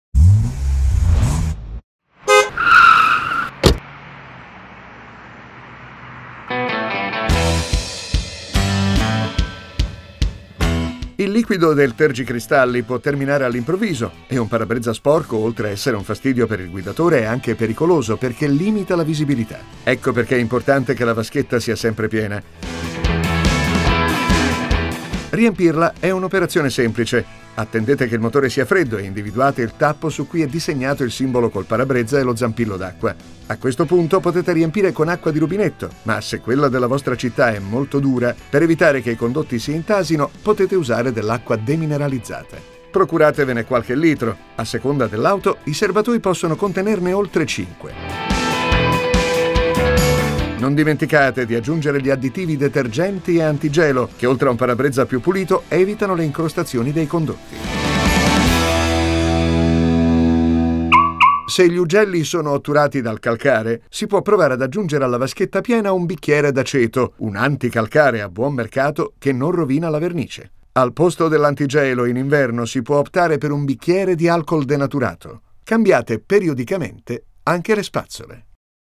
Come si rabbocca l'acqua dei tergicristalli
Un parabrezza sporco, oltre ad essere un fastidio per il guidatore, è anche pericoloso perché limita la visibilità. Per questo è importante non rimanere senza liquido del tergicristalli. La vaschetta dovrebbe essere sempre piena e per assicurarsi di poterla rabboccare anche durante un lungo viaggio è bene tenere nel portabagagli, piuttosto che nell'abitacolo per non dare fastidio ai passeggeri o peggio ritrovarsela sotto ai pedali mentre si guida, una bottiglia d'acqua (o almeno una bottiglia vuota da riempire all'occorrenza). Questa accortezza non sempre garantisce di poter riempire del tutto la vaschetta, dato che le sue dimensioni variano di modello in modello fino a superare i cinque litri, ma in caso di necessità può garantirvi vetri puliti per una buona visibilità. [...]